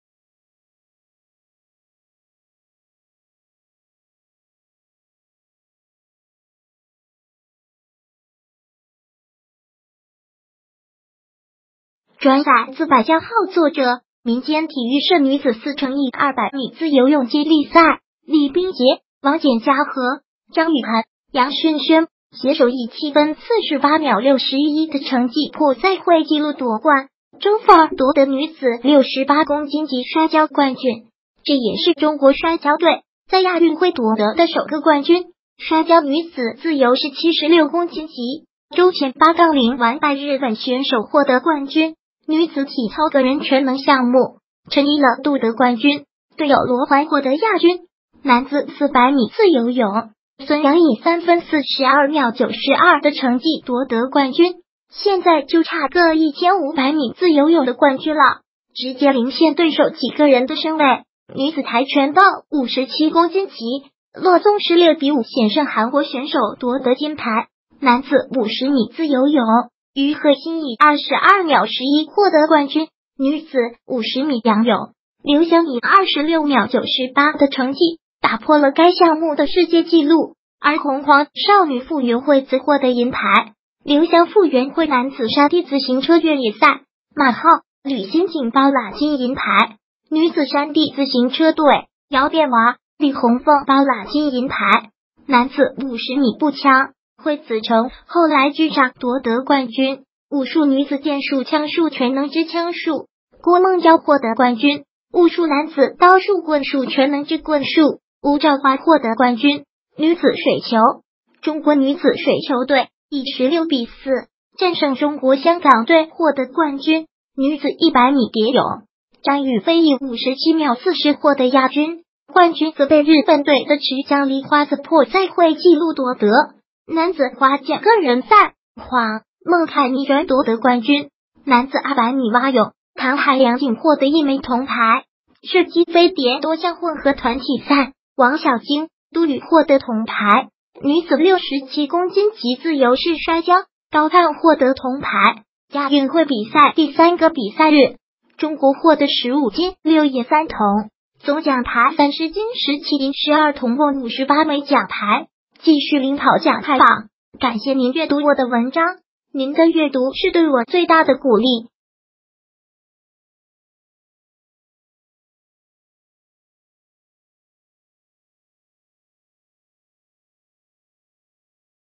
亚运会比赛第三日，刘湘破世界纪录！中国15金6银3铜！继续领跑！
转载自百家号作者：民间体育社女子4*200米自由泳接力赛，李冰洁、王简嘉禾、张雨涵、杨浚瑄携手以7分48秒61的成绩破赛会记录夺冠。周凤夺得女子68公斤级摔跤冠军，这也是中国摔跤队在亚运会夺得的首个冠军。摔跤女子自由式76公斤级，周倩8-0完败日本选手获得冠军。女子体操个人全能项目，陈一乐夺得冠军，队友罗欢获得亚军。男子400米自由泳，孙杨以3分42秒92的成绩夺得冠军，现在就差个1500米自由泳的冠军了。直接陵县对手几个人的身位。女子跆拳道57公斤级，骆宗诗6-5险胜韩国选手夺得金牌。男子50米自由泳，余贺新以22秒11获得冠军。女子50米仰泳，刘湘以26秒98的成绩打破了该项目的世界记录，而洪荒少女傅园慧则获得银牌。刘湘傅园慧男子山地自行车越野赛，马皓、吕先景包揽金银牌。女子山地自行车队，姚变娃、李洪凤包揽金银牌。男子50米步枪，惠子程后来居上夺得冠军。武术女子剑术-枪术全能之枪术，郭梦姣获得冠军。武术男子刀术/棍术全能之棍术，吴照华获得冠军。女子水球，中国女子水球队以16-4战胜中国香港队获得冠军。女子100米蝶泳，张雨菲以57秒40获得亚军，冠军则被日本队的池江璃花子破赛会记录